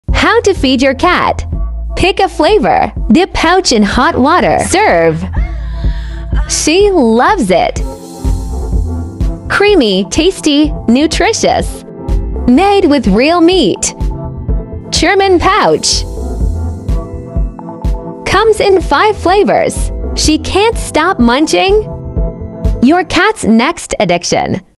Cherman Wet Cat Food
Cherman Cat Pouches are filled with the delicious taste your cat will love and enjoy. Give your Cat Cherman, your Cat's next addiction.